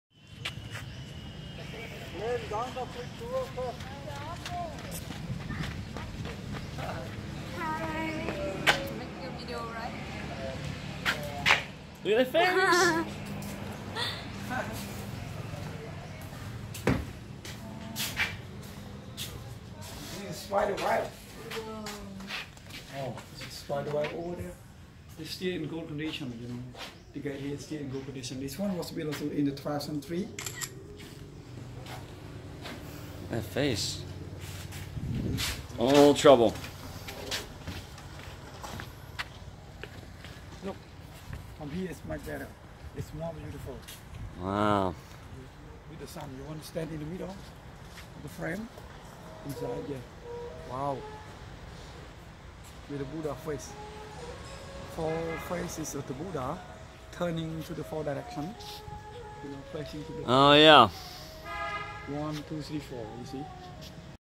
4 Faces of Buddha Welcome us to Cambodia Ancient Ruins Banteay Kdei with Michael S Novilla
Today is Wednesday June 27, 2018 and I'm in Krong Siem Reap, Cambodia, on a terribly hot, humid, day long tour visiting Ancient Cities, Temple Pagodas and Library Ruins.

Here we are visiting Banteay Kdei, "Unrestored historic temple and Buddhist monastery built of sandstone, with sculptures and a moat." (Google Maps ) and is located near the "7 Wonders of the World" infamous Angkor Wat. Angkor = City , Wat = Buddhist Pagoda. Check out the 4 faces of the Buddha, facing North, South, East and West,

Banteay Kdei
Angkor Archaeological Park, Siem Reap, Cambodia

Shot in one, unedited take, on my Galaxy Note 8 Smartphone